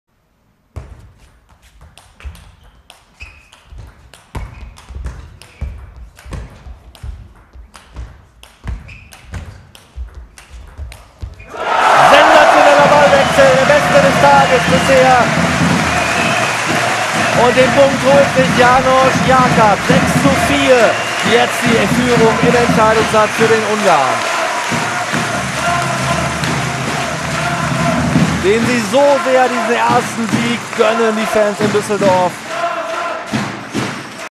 Point Of The Week - Janos Jakab HNT
Elkezdjük heti rendszerességgel megosztani a válogatott játékosok legszebb poénjait! Ezt a "point of the week" szöveggel fogjuk feltüntetni!